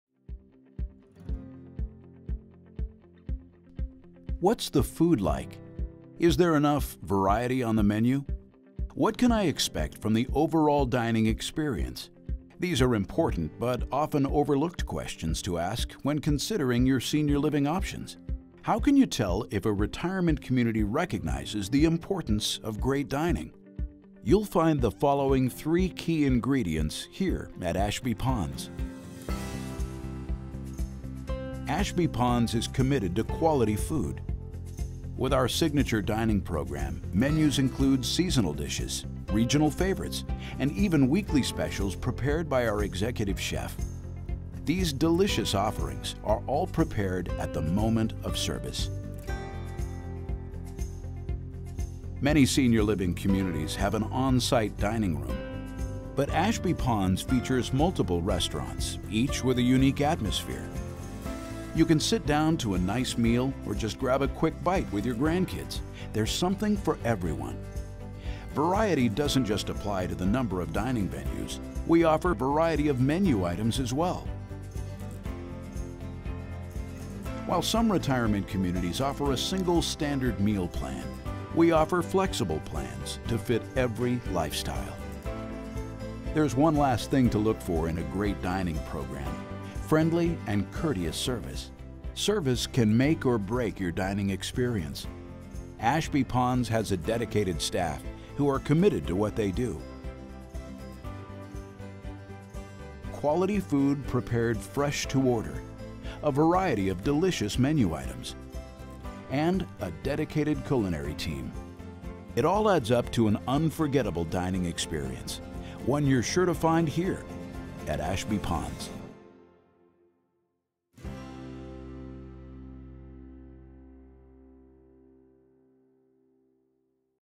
Ashby Ponds Senior Living: What is the dining experience like?
What's the food like? Is there enough variety on the menu? What can I expect from the overall dining experience? It is important to learn as much as you can about the dining program when considering a senior living community. At Ashby Ponds in Ashburn, VA, dining is a quintessential element of the senior lifestyle. Watch as members of our culinary team explain the factors to consider to ensure your retirement community offers an exemplary dining experience.